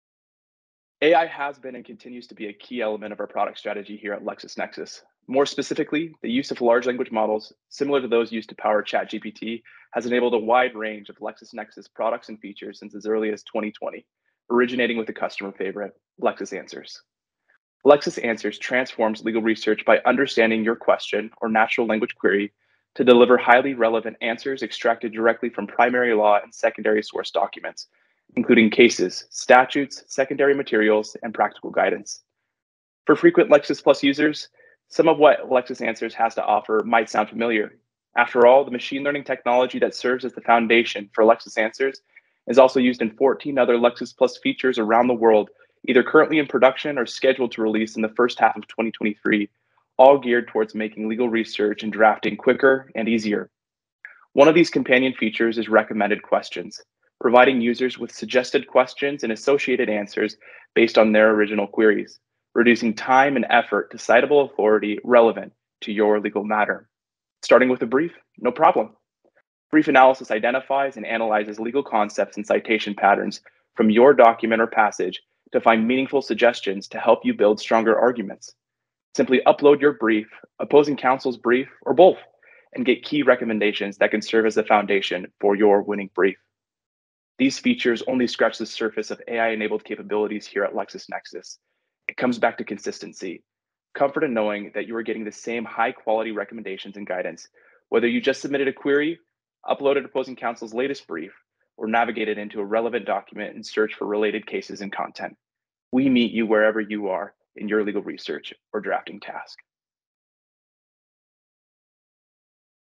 Why LexisNexis is the leader in Legal AI
Jake Nelson discusses why LexisNexis has always been a leader in Legal AI, with machine learning technology, natural language processing and large language models powering some of our most innovative features in Lexis+!